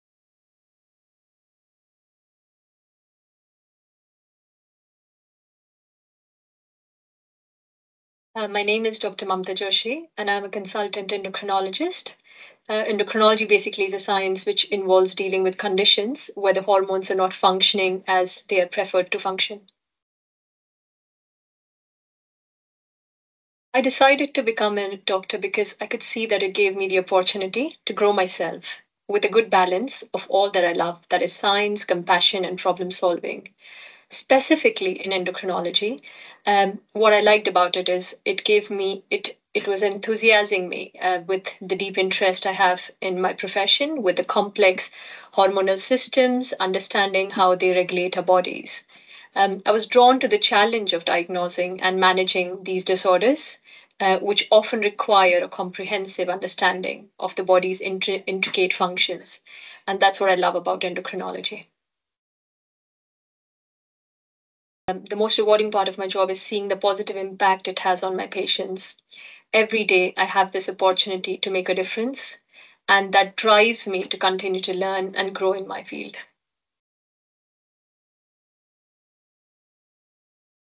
An introduction to Dr Mamta Joshi
Leading endocrine consultant Dr Mamta Joshi tells us all about her work, why she chose to become a doctor and what she finds to be the most rewarding aspect of her job, in this introductory video.